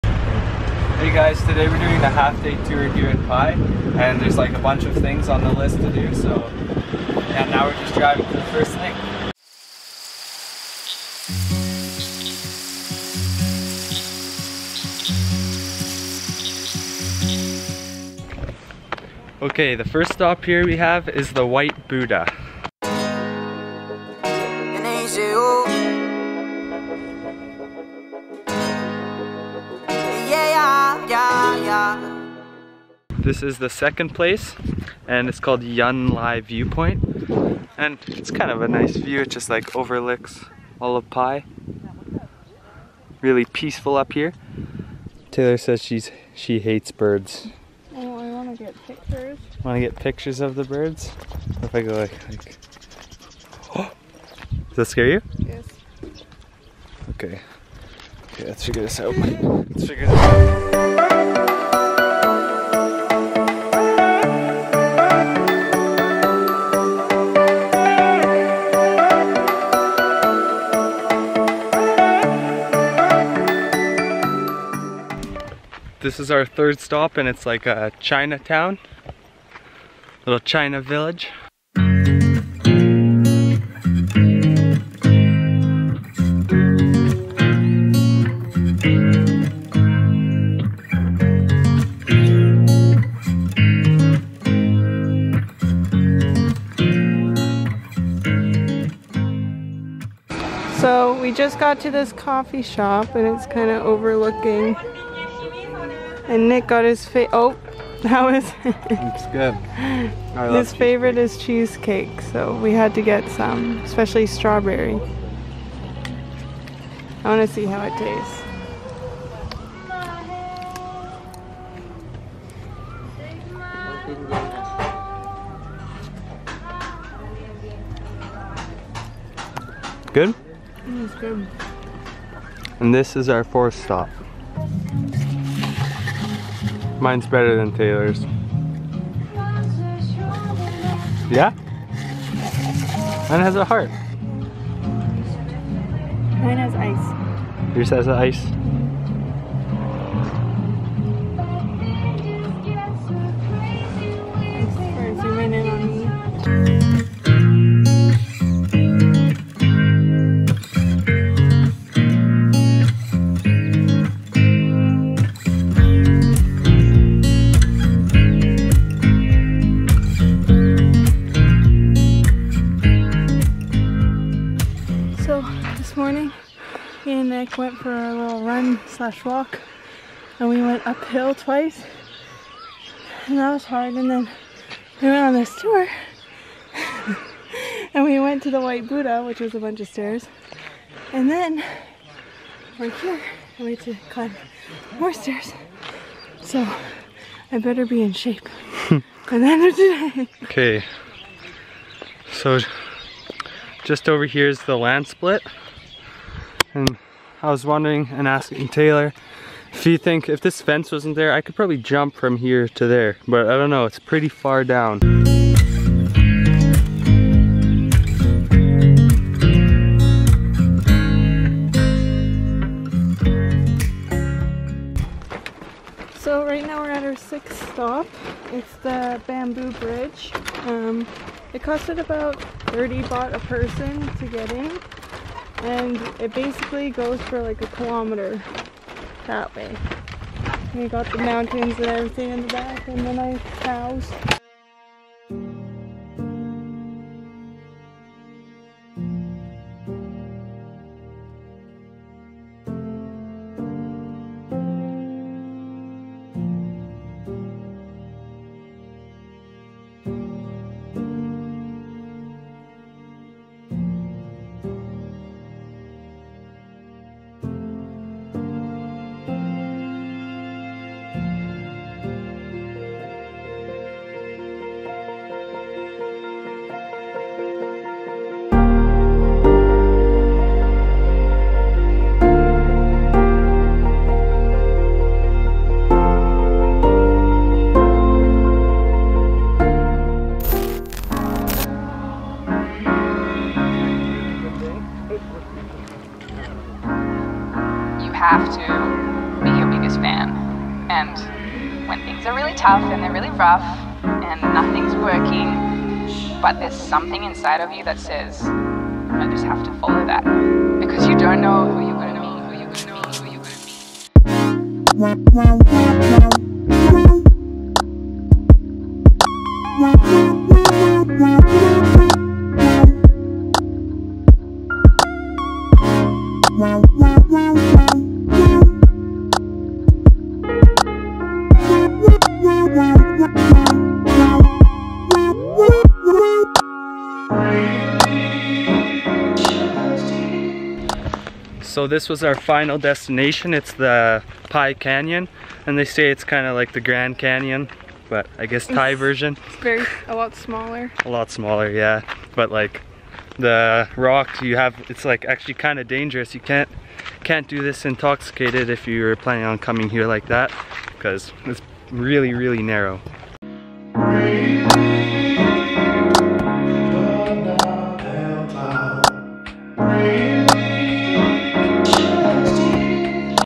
MUST SEE places in Pai
We did almost every main tourist attraction in the small hippie town of Pai, Thailand and this is what it looked like.